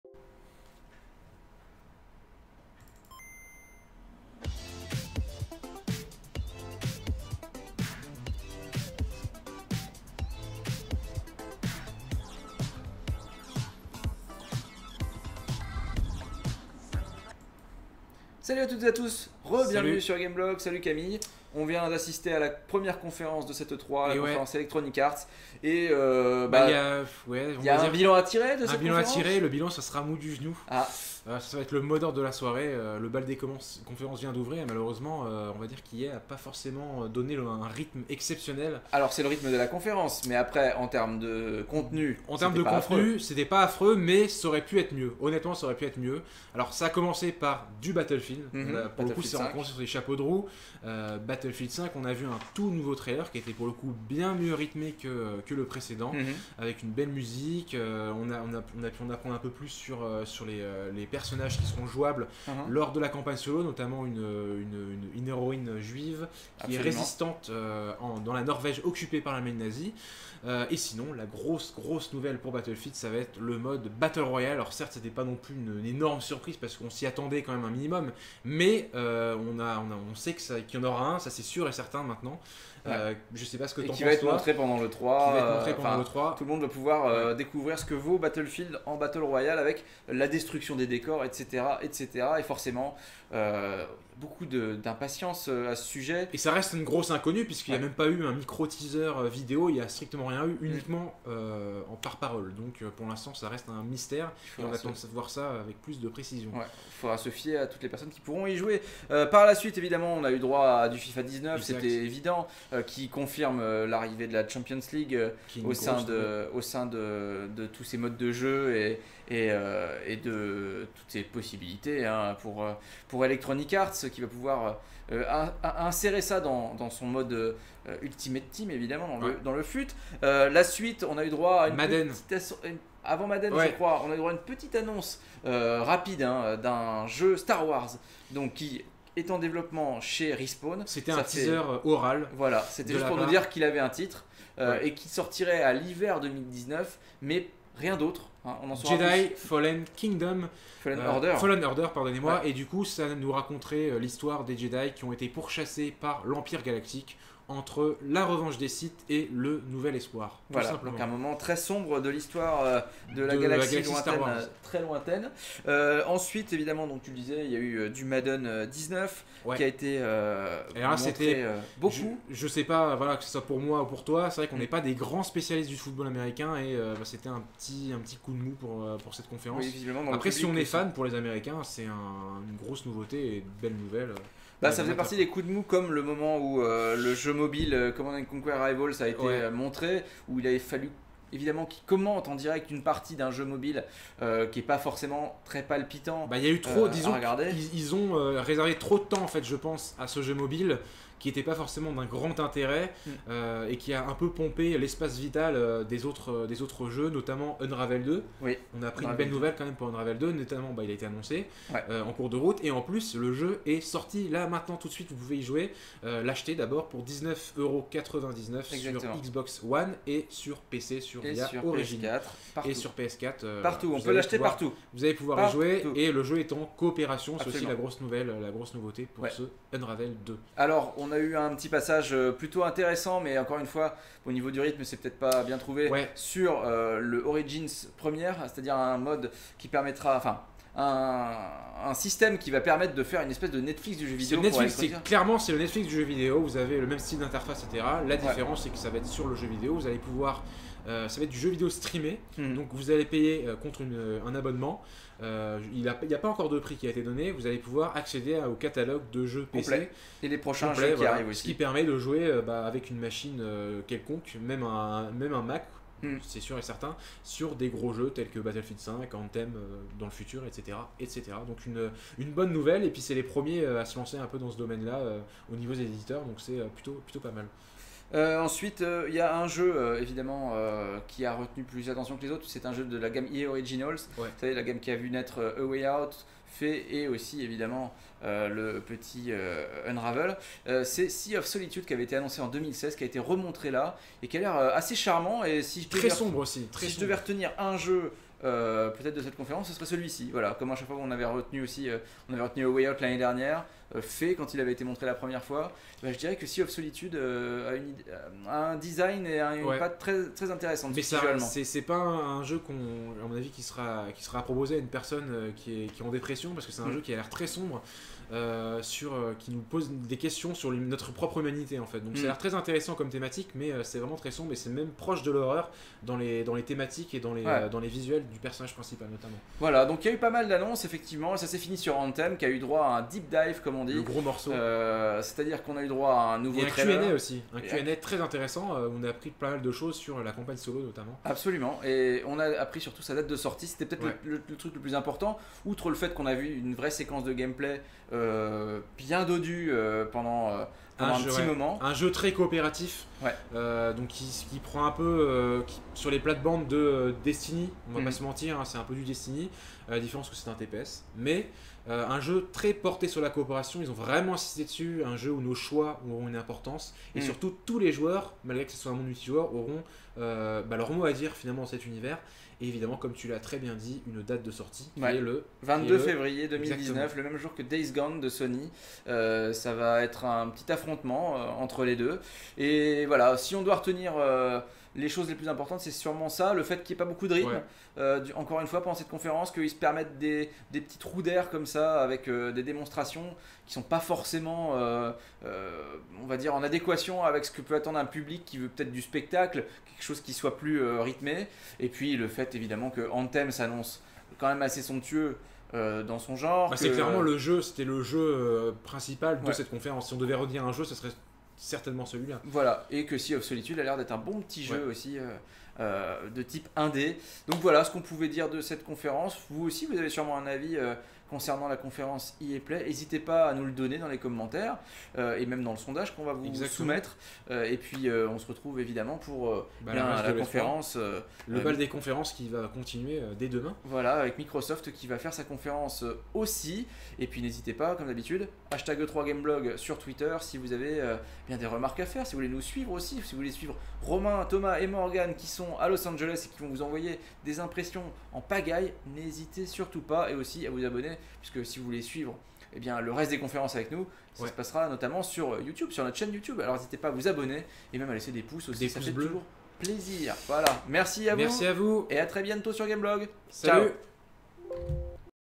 E3 2018 : On débriefe la conférence EA Play
Que retenir de la conférence E3 2018 d'Electronic Arts ?